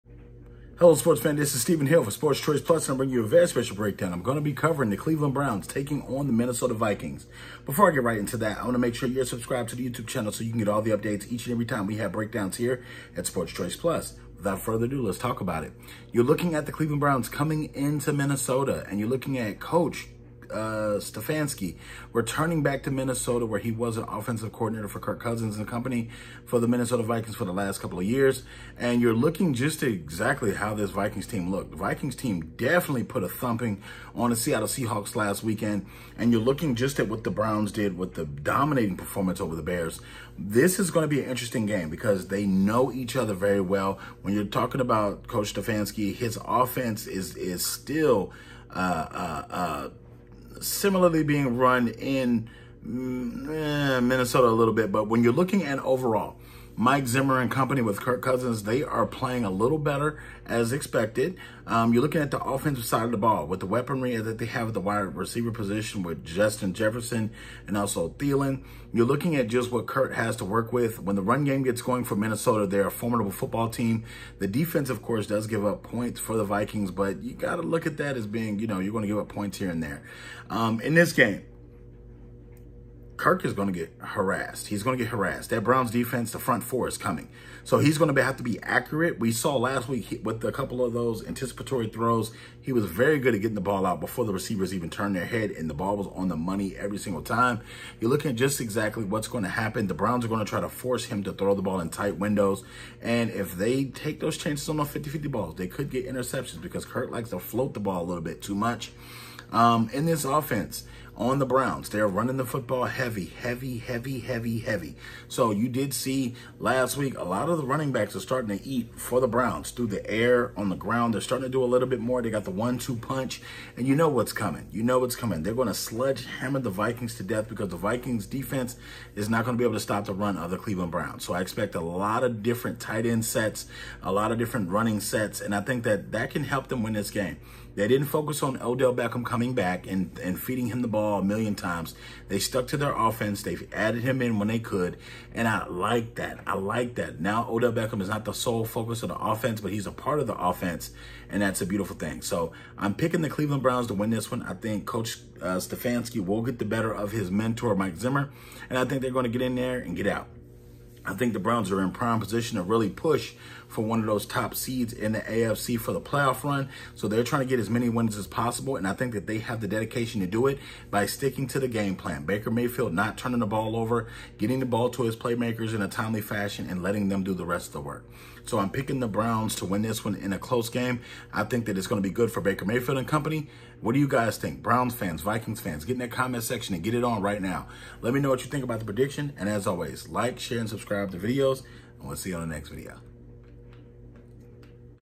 NFL Week 4 2021-2022 Cleveland Browns vs Minnesota Vikings
The Vikings host the Browns in week 4 of the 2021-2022 NFL season. Also click the link for the facebook